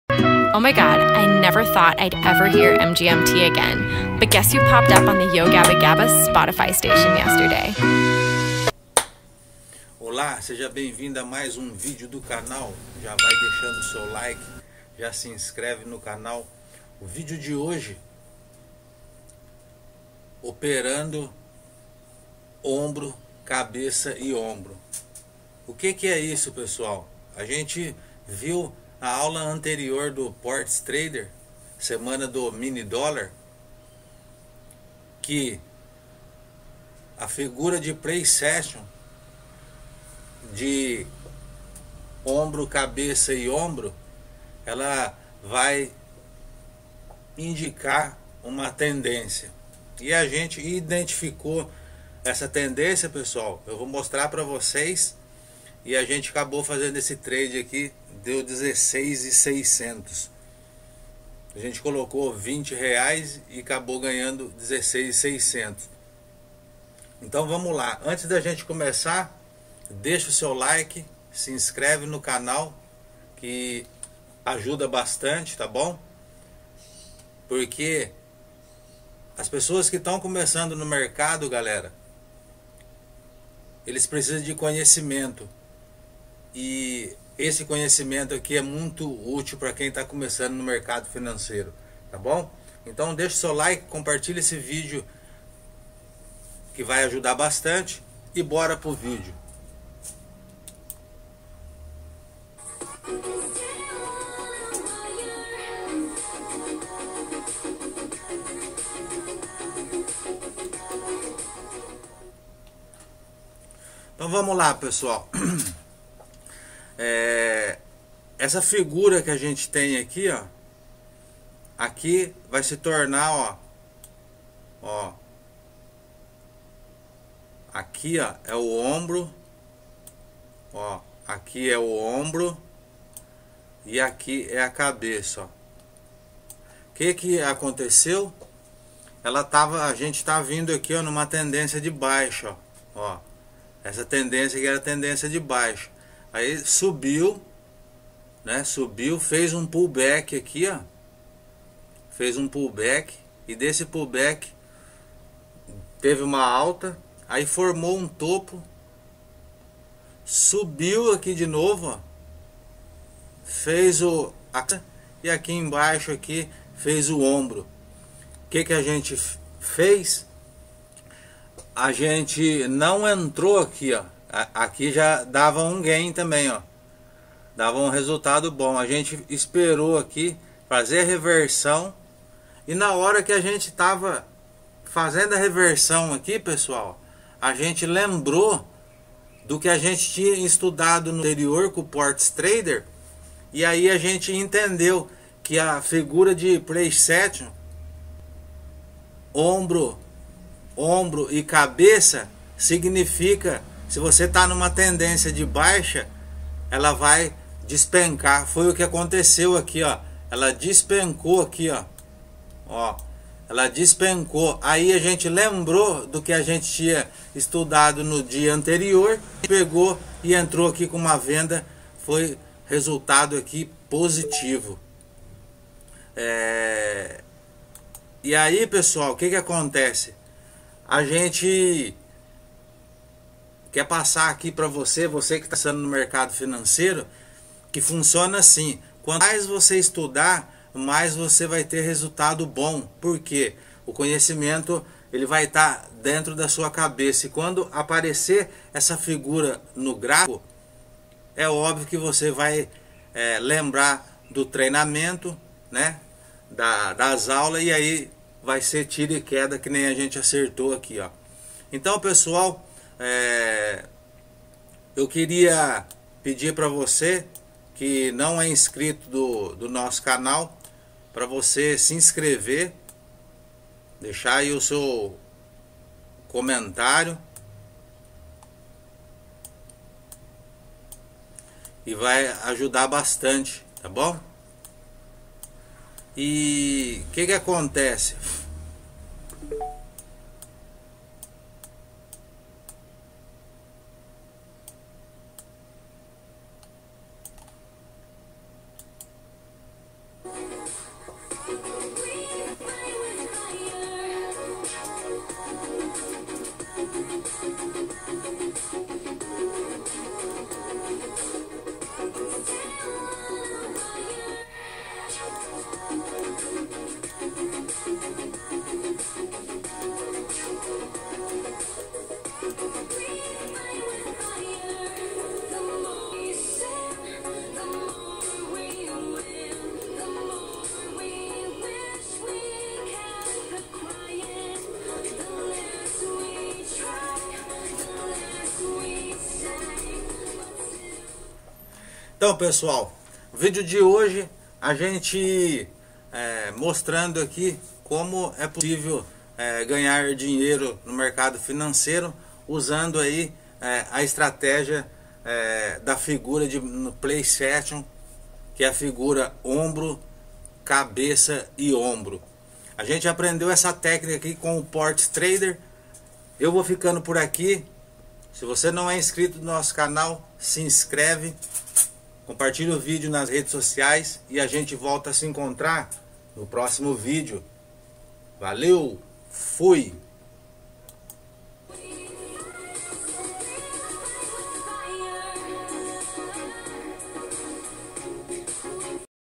FIGURA DE PRICE ACTION NA TENDENCIA DE BAIXA É GAIN NA OPÇÕES BINARIAS USD/JPY
Neste vídeo ,coloquei em pratica a figura de price action na tendencia e fiz um gain . Obrigado ao professor Ports Trader .
Espero que vocês curtem o video